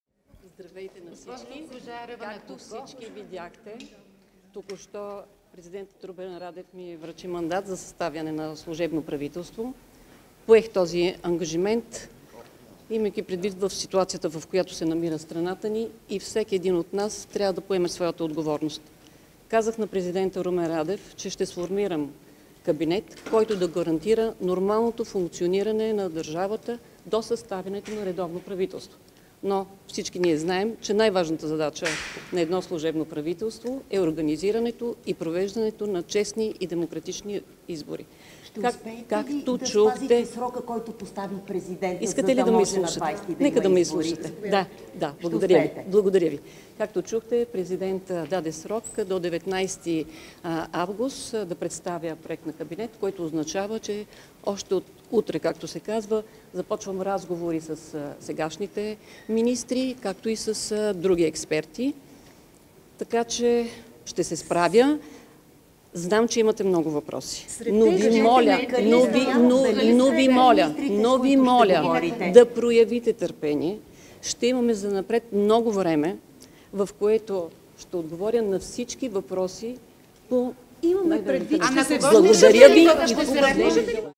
Още от утре започвам разговори със сегашните министри и други експерти, каза Горица Кожарева
Още от утре започвам разговори със сегашните министри и други експерти, каза кандидатът за служебен премиер Горица Грънчарова-Кожарева пред журналисти, след като се срещна с президента Румен Радев.

Ще сформирам кабинет, който да гарантира нормалното функциониране на държавата до съставяне на редовно правителство, каза тя. Кожарева подчерта, че най-важната задача на служебното правителство е провеждането на честни избори.

Поех този ангажимент, имайки предвид ситуацията, в която се намира страната ни, и всеки един от нас трябва да поеме своята отговорност, заяви Кожарева. Най-важната задача на едно служебно правителство е организирането и провеждането на честни и демократични избори, добави тя. Президентът даде срок до 19 август да представя проект на кабинет, което означава, че още от утре започвам разговори със сегашните министри, както и други експерти, така че ще се справя, заяви кандидатът за служебен премиер.

Горица Грънчарова-Кожарева отказа да отговаря на въпроси на журналисти, помоли за търпение и посочи, че занапред ще има много време, в което ще отговори на всички въпроси.

Репортери - Николай Трифонов, Елена Николова
Оператор - Красимир Михайлов